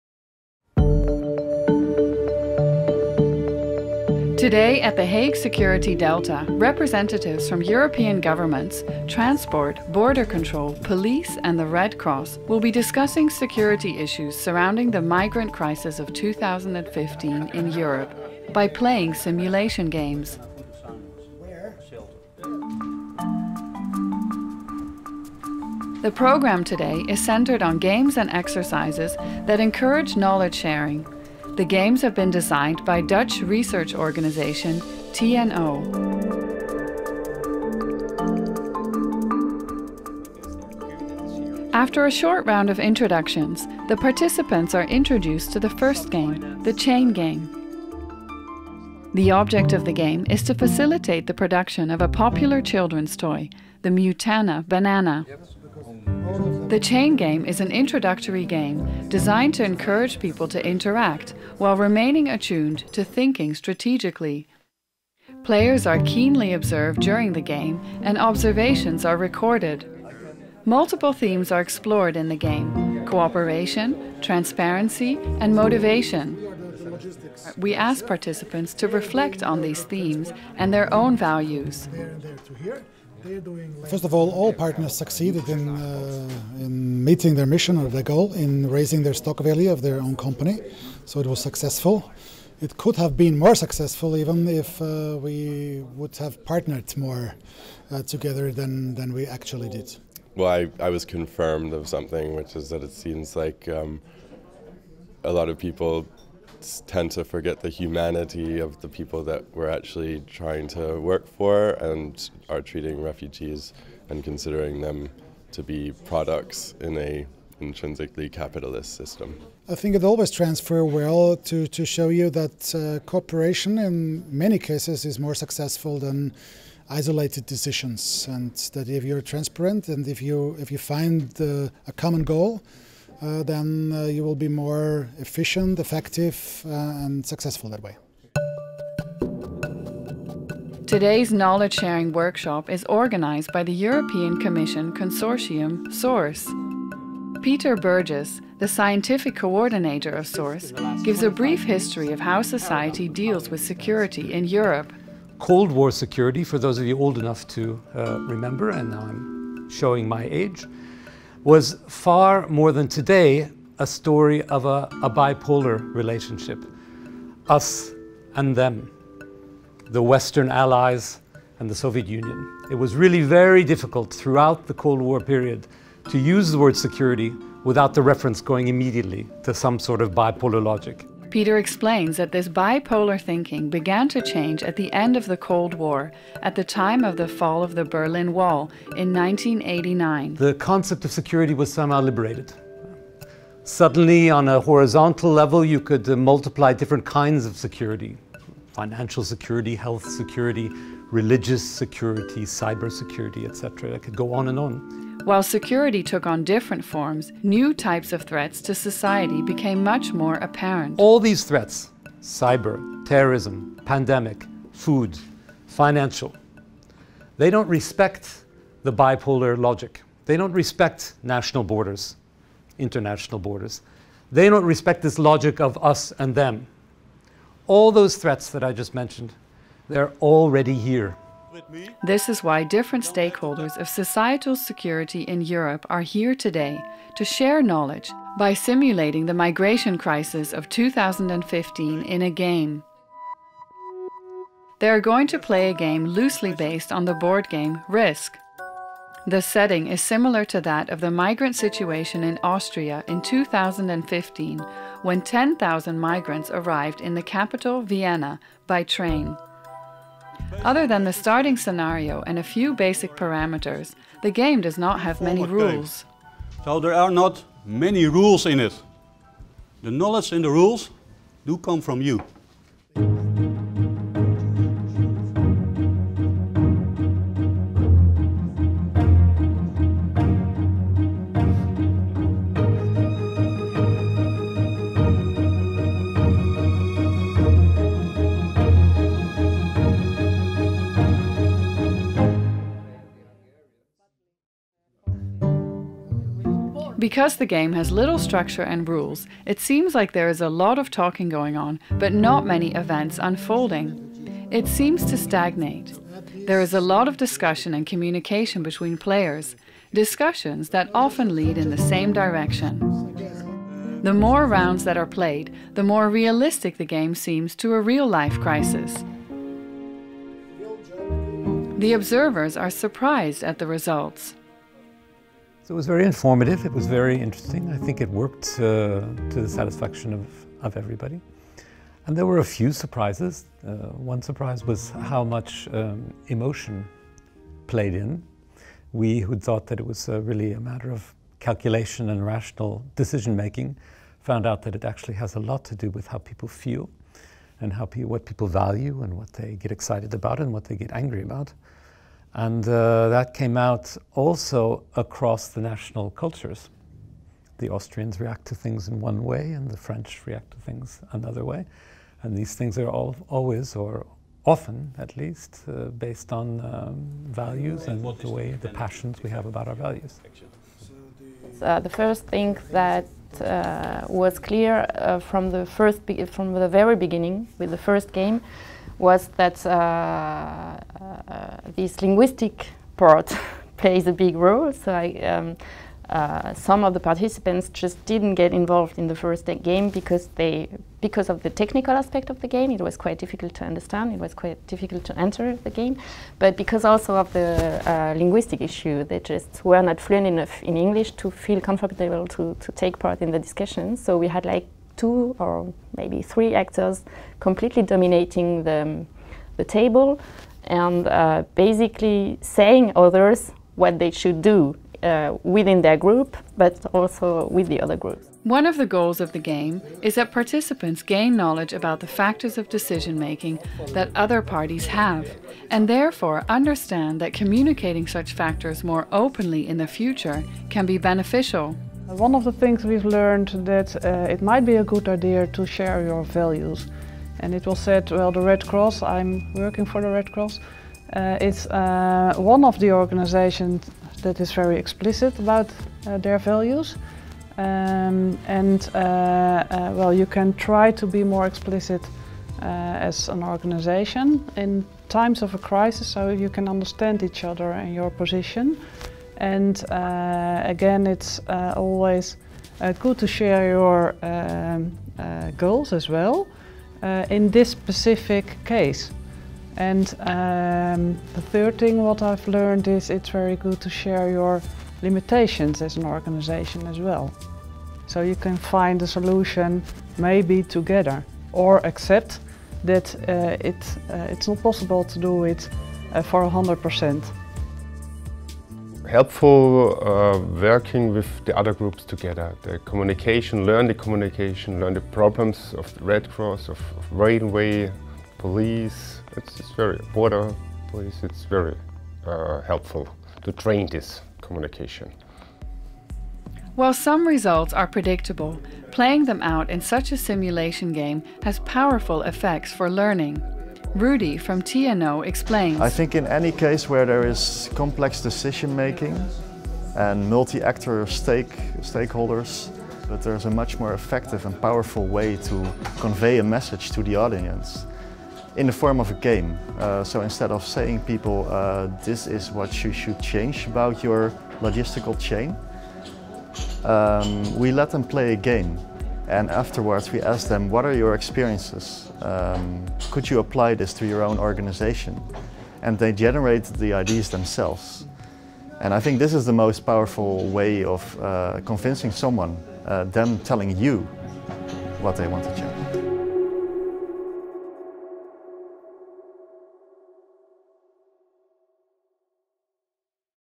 Source, red de excelencia para la promoción de aspectos sociales de la seguridad (audio en inglés)
SOURCE, la red de excelencia dedicada al debate y promoción de los aspectos sociales de la seguridad, organizó un taller sobre la inmigración ilegal transeuropea con diversos agentes relacionados con la gestión de los flujos migratorios que incluían policía de fronteras, miembros de la administración, y ONGs, procedentes de países como Austria, Alemania, Países Bajos, Francia y España.

El taller se celebró en las instalaciones de Security Delta en La Haya, que gestiona TNO, miembro del consorcio, y que también contó con la participación de TECNALIA como coorganizadora.
La sesión consistió en dos instancias de serious gaming, el primero de ellos de temática diferente del tema central, destinado a romper el hielo entre los asistentes y reforzar el espíritu colaborativo, y el segundo relacionado con un caso de inmigración ilegal similar al que se produjo en las fronteras de Austria, Hungría y Alemania en 2014. Los agentes implicados debatieron el proceso conjunto de toma de decisiones y cómo la falta de un objetivo político concreto llevó de hecho a una indefinición de las acciones.

En una sesión de contraste posterior se determinó que las culturas organizativas de las instituciones presentes redundaban en que fuera complicado tomar decisiones conjuntas, en especial, en caliente o en teatro de operaciones. Sí que se valoró de modo positivo que se llevaran a cabo ejercicios de serious gaming entre los agentes implicados, al menos para conocimiento mutuo y mejorar las relaciones en caso de crisis.

Con este taller, SOURCE termina su actividad destinada a indagar sobre técnicas apropiadas para la colaboración de los implicados en temas de seguridad que conlleven un mejor entendimiento mutuo y la inclusión de los aspectos sociales en las acciones conjuntas. TECNALIA  ha dirigido un world café sobre amenazas futuras en 2015 y un round robin brainstorming sobre la seguridad de IoT en 2017, y ha sido autora del informe final que se entregará a la Comisión este año. Una de las participantes en el taller de La Haya proviene de la colaboración en materia de Seguridad entre TECNALIA y el Real Instituto Elcano.

Para más información:
Javier Herrera, Área de Óptima/Optimization, Modelling & Analytics